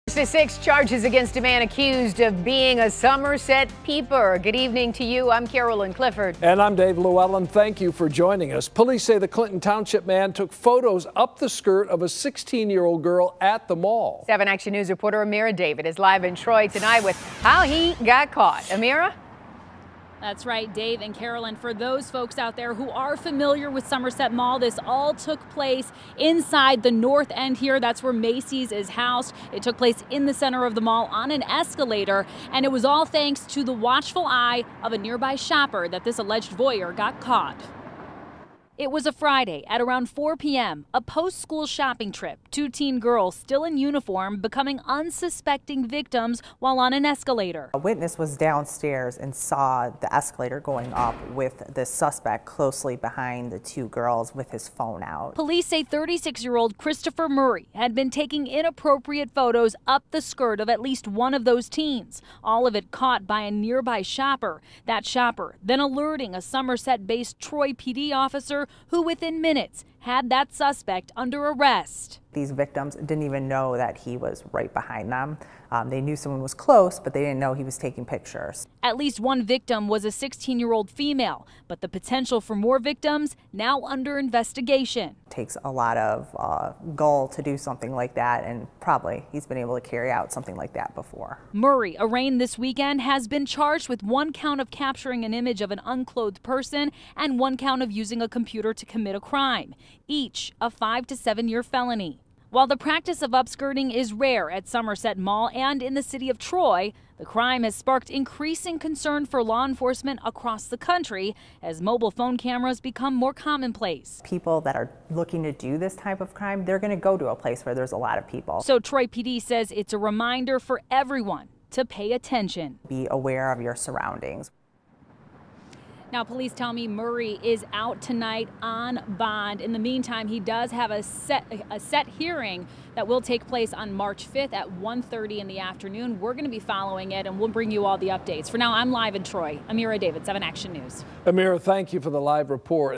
Man arrested for allegedly taking upskirt photos of teen at metro Detroit mall
Troy police say a man is in custody for allegedly taking upskirt photos of a teen inside the Somerset Collection on Friday.